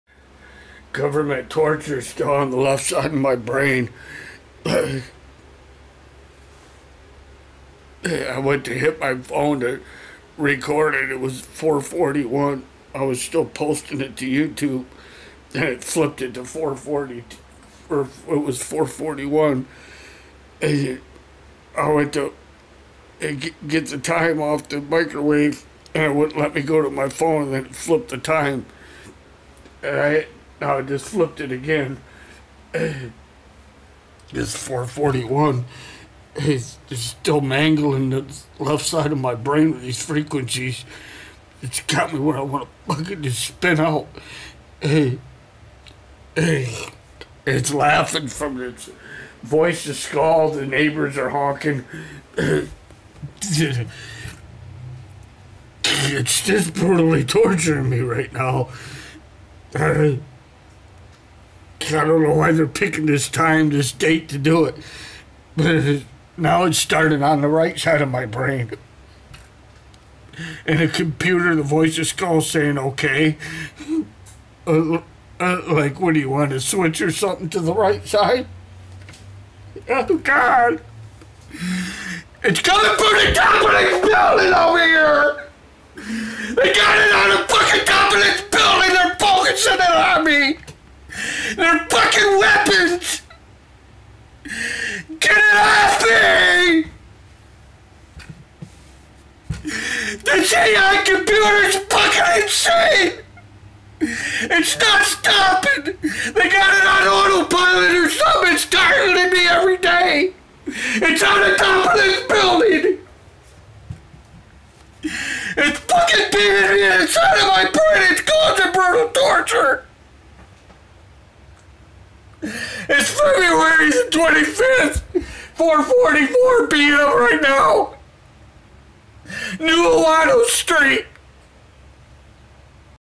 4:44pm. NUUANU ST TARGETING WEAPON! VOICE OF GOD WEAPON!
BRUTAL TORTURE FROM THE MICROWAVE TOWER ON TOP OF BIULDING NUUANU ST. EXTREME FOCUSED RADIO MICROWAVE FREQUENCIES MESSINGS MY BRAIN UP! SEVERE TORTURE!